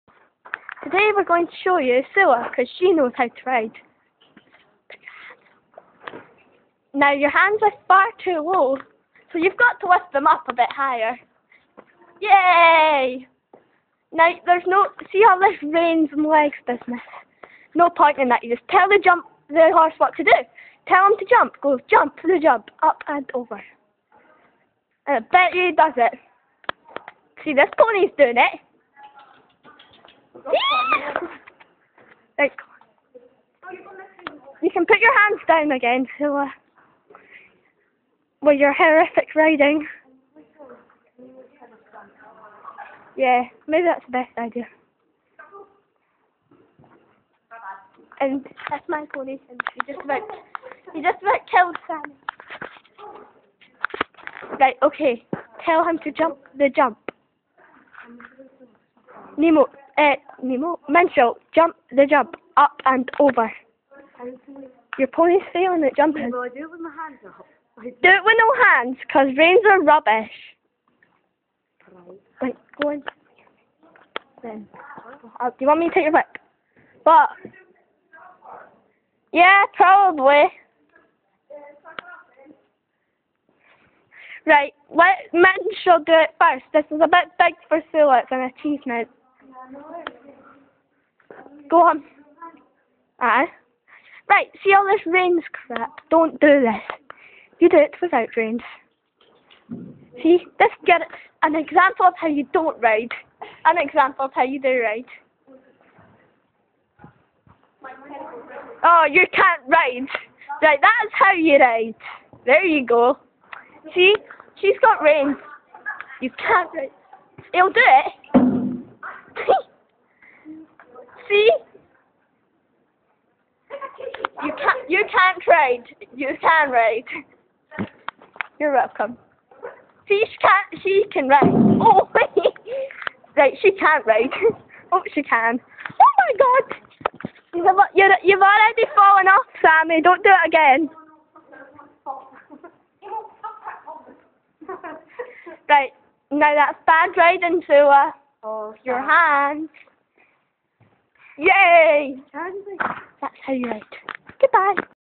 This Is How You Ride-Part One!
READDD:
this is pisstake video for all the critiquers!
So Me Sula And Sammie decided to make this to show how you ride:)!we are gonna do a follow up on this sooon:)!donno when,but soon!

GOOODBYE.